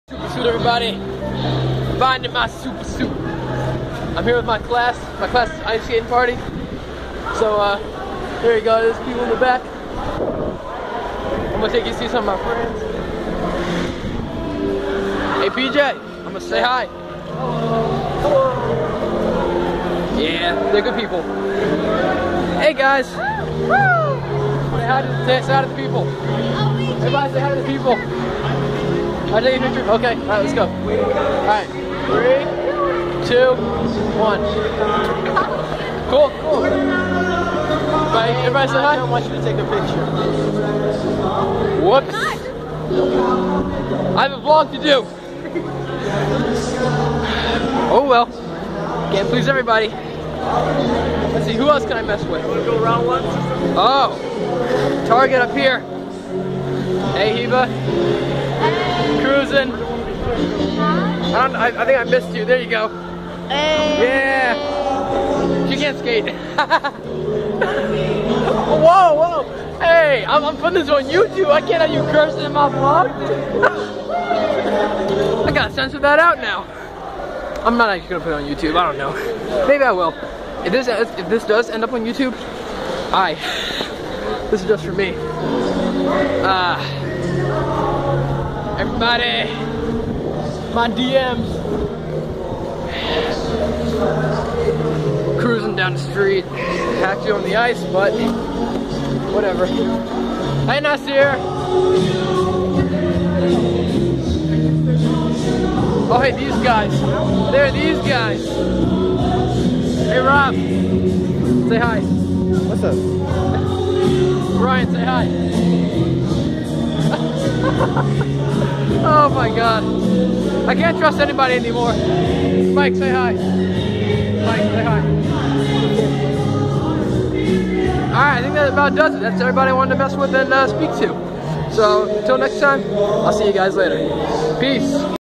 275 Ice Skating Party
A vlog from two years ago. Just thought I'd post it cus it's been sitting in my phone and I need space.